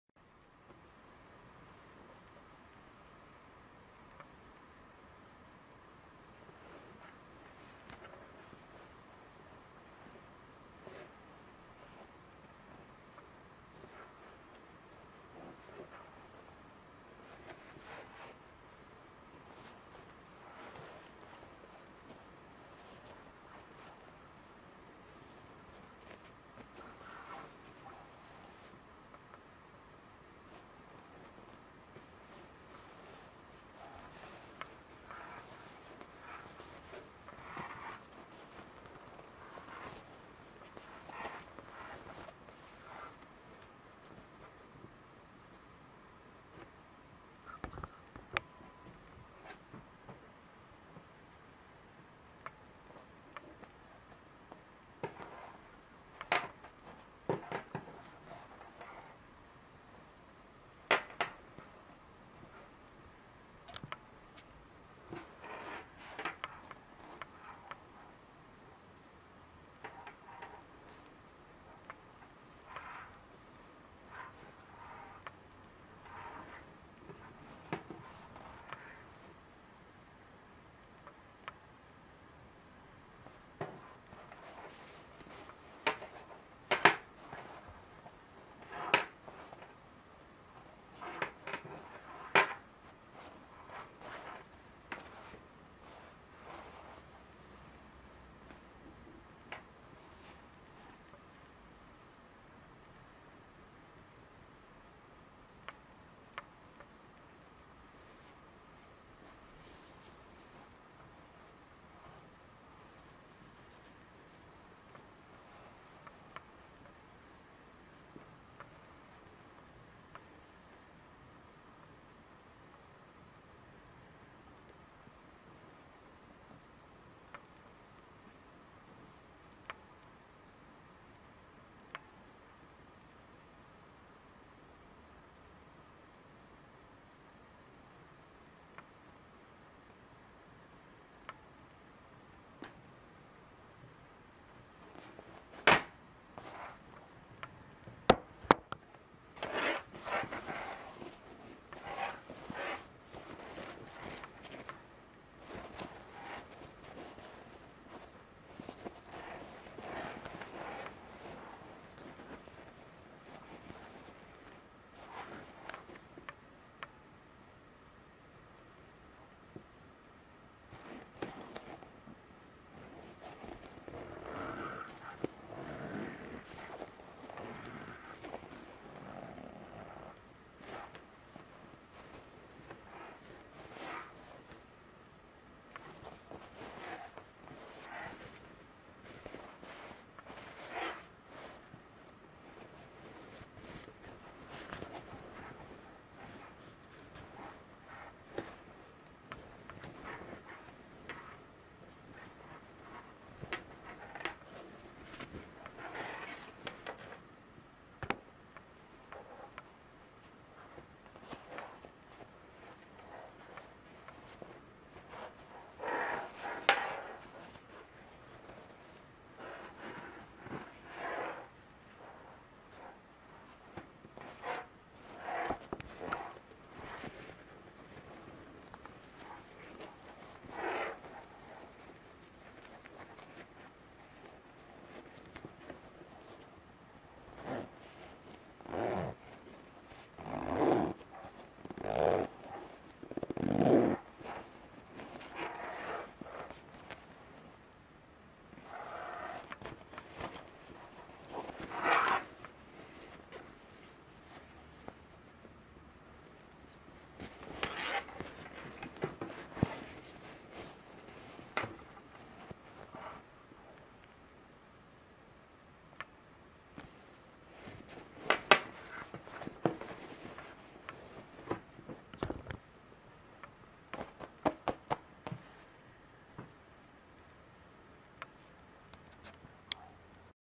Hamburger Backpack Bag Childrens School Bag Burger Bag Collectors item Review
Hamburger backpack review. You can't buy these around here.
Collectors item.